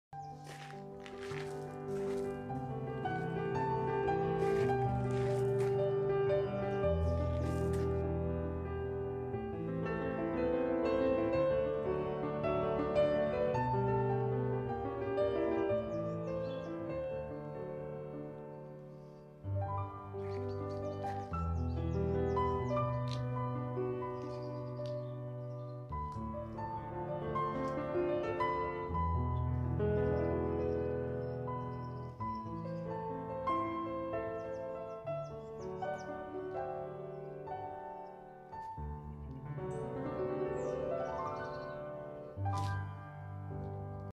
This Victorian shawl is HUGE
I've been working from an original Victorian knitting pattern to create this 1840s shawl. It's huge, but it's still not quite complete.